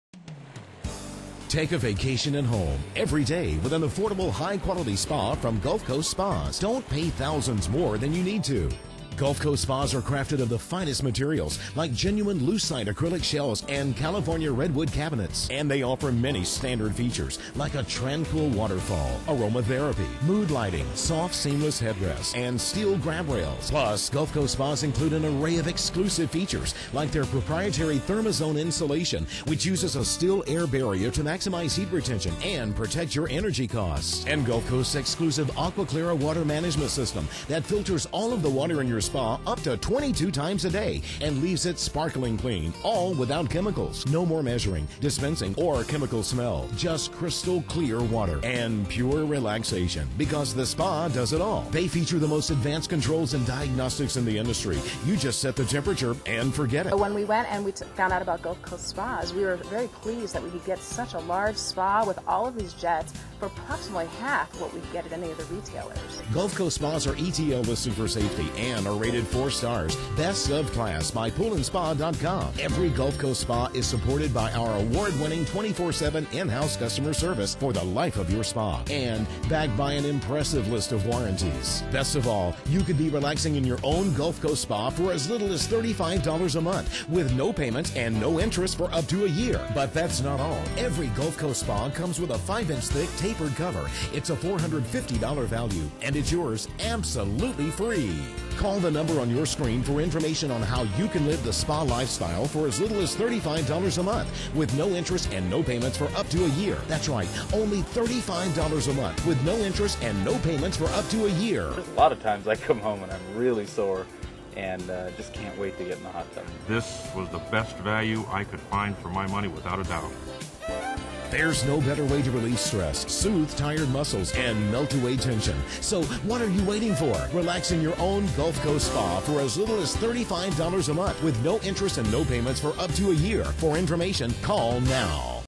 Gulf Coast Spas Commercial
A segment from the full length informercial we shot for Gulf Coast Spas.